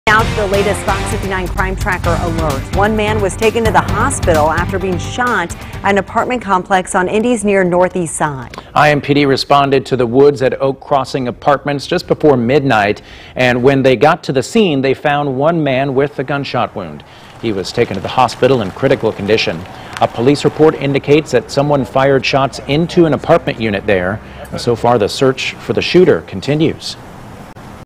Man in critical condition after shooting at northwest side apartment complex
The Indianapolis Metropolitan Police Department is investigating a shooting in the Woods at Oak Crossing Apartment complex in Indy's Northwest side.